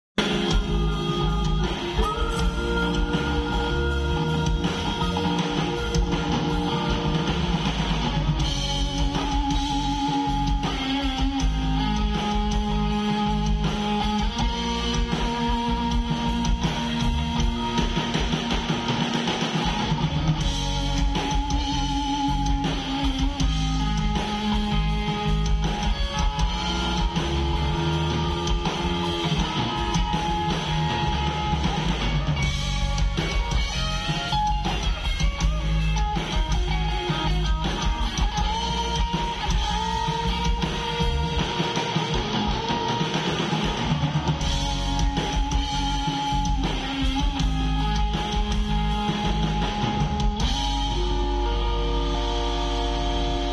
Blenders rozgrzewka Pinta Party 2024
próba dźwięku Blenders Pinta_Party_2024 Wieprz festiwal piwa